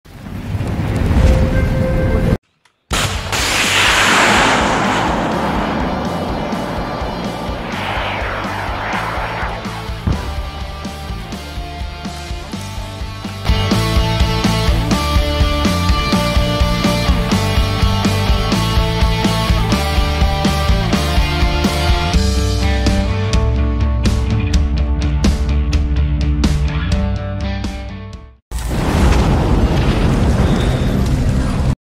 Січеславські десантники знищили черговий танк російських окупантів🔥
🔺Протитанковим підрозділом 25-ї окремої повітрянодесантної Січеславської бригади ДШВ ЗС України успішно знищено черговий танк противника. Продовжуємо й далі вести бойові дії та захищати Батьківщину.

Відео: Командування ДШВ ЗСУ